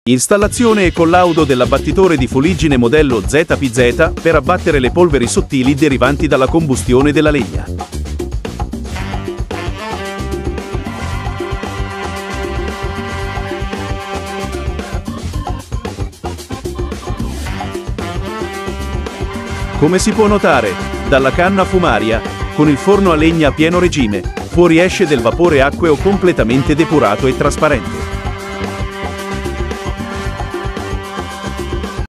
Abbattitore di Fuliggine mod. ZPZ per forno a legna
IT - Installazione e collaudo dell’abbattitore di fuliggine modello ZPZ per abbattere le polveri sottili derivanti dalla combustione della legna. Come si può notare, dalla canna fumaria, con il forno a legna a pieno regime, fuoriesce del vapore acqueo completamente depurato e trasparente! 💨 💨 Un altro passo verso la sostenibilità!🌍

EN - Installation and testing of the ZPZ model soot collector to abate fine dust from wood combustion. As you can see, completely purified and transparent water vapour escapes from the chimney with the wood-burning oven at full capacity! 💨 💨 Another step towards sustainability! 🌍